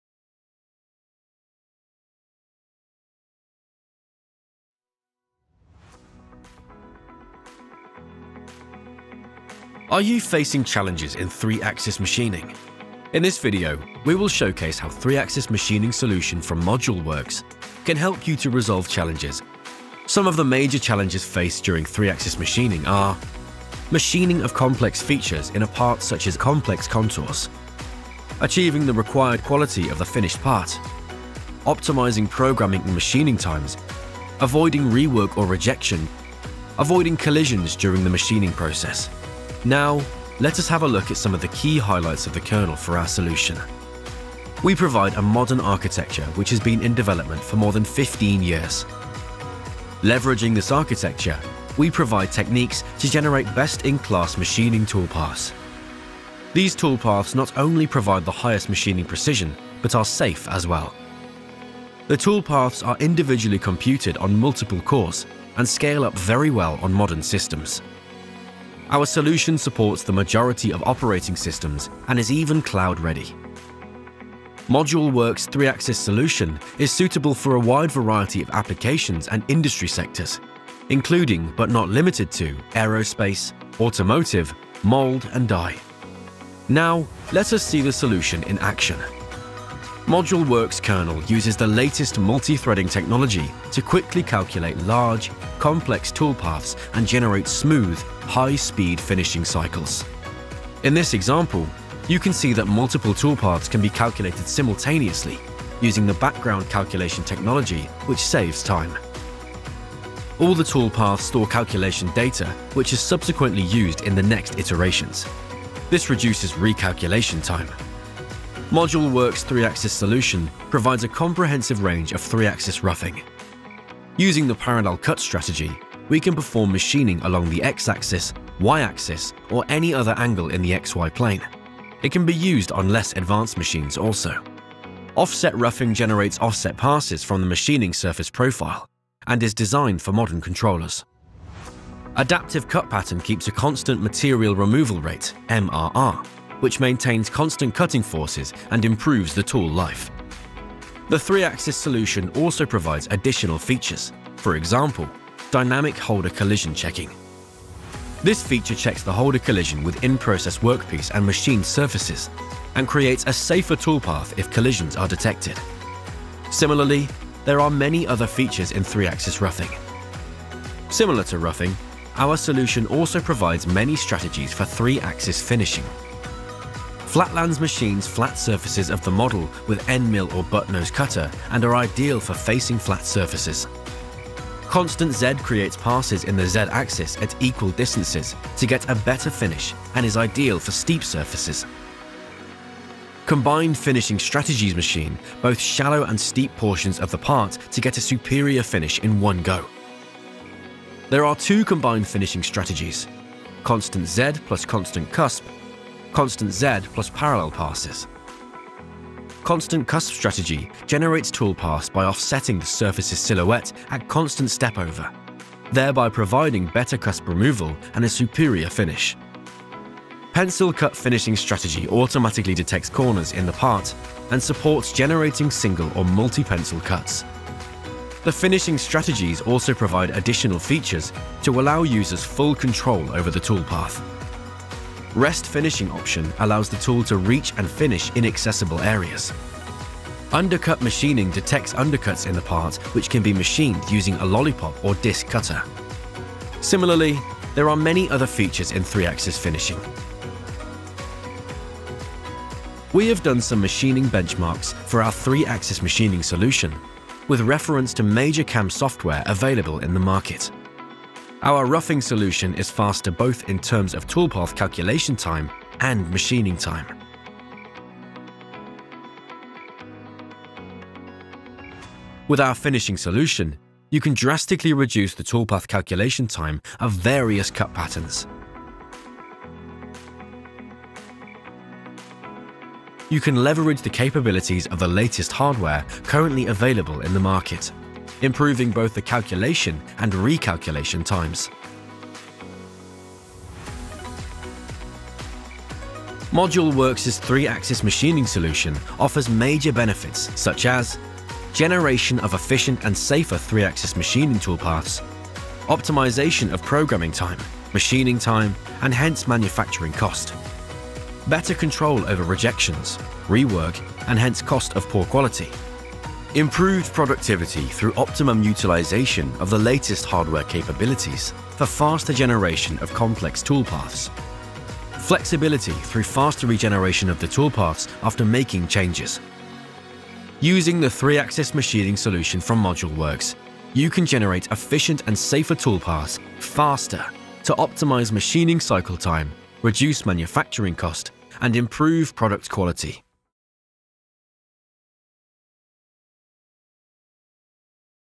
3-Axis Machining by ModuleWorks
Are you facing challenges in 3-Axis machining? In this video, we will showcase how 3-Axis Machining Solution from ModuleWorks can help you to resolve those challenges and optimize your process.